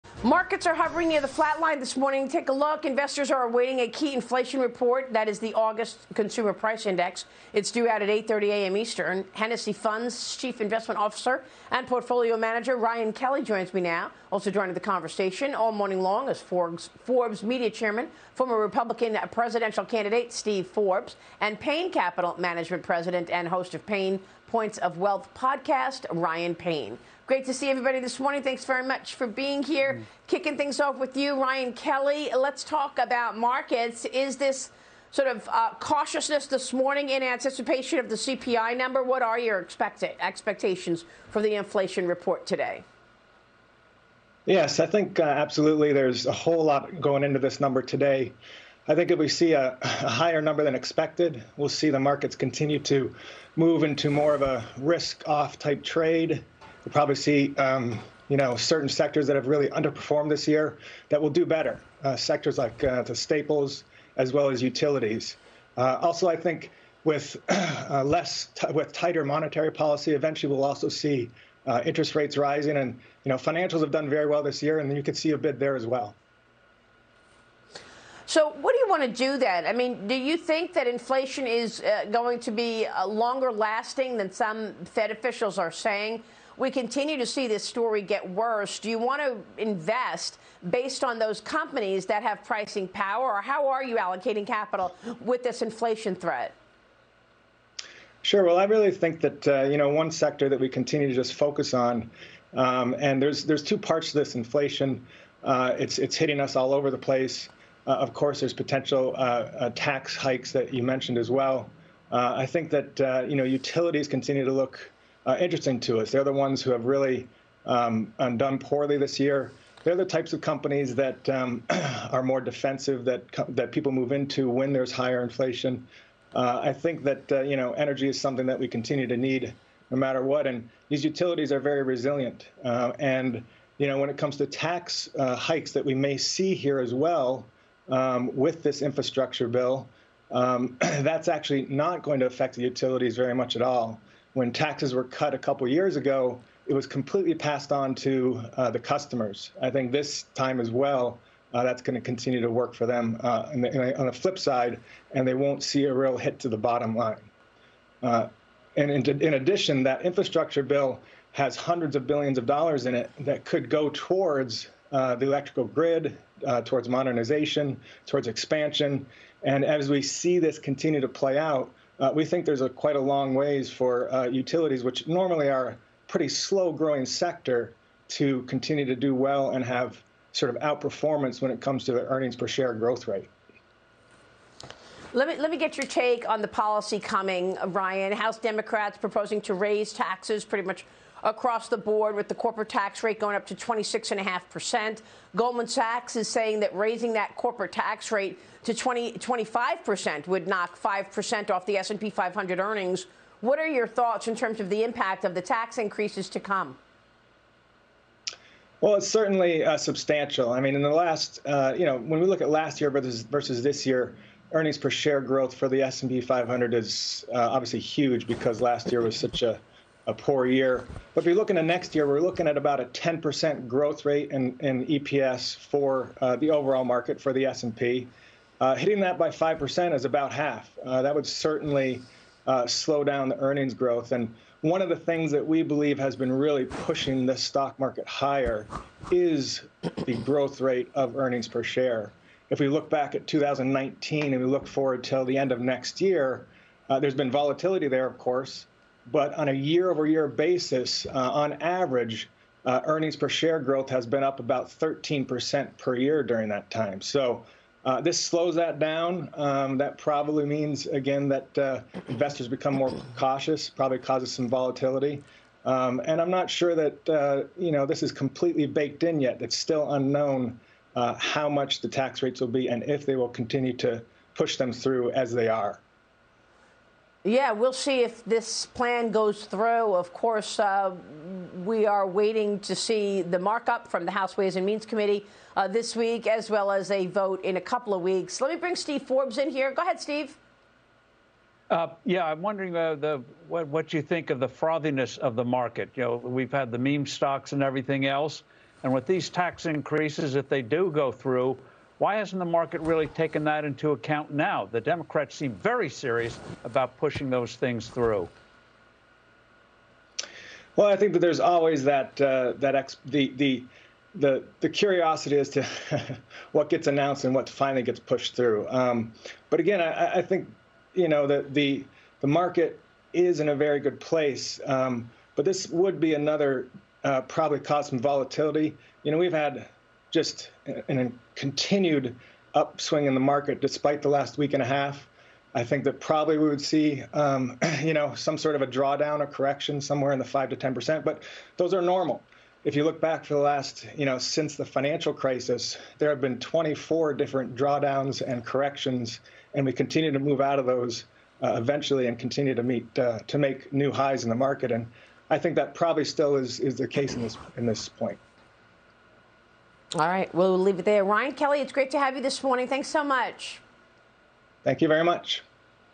Portfolio manager says these stocks are resilient to inflation
Hennessey Funds CIO and portfolio manager Ryan Kelley says utilities may outperform their earnings per share growth rates in the market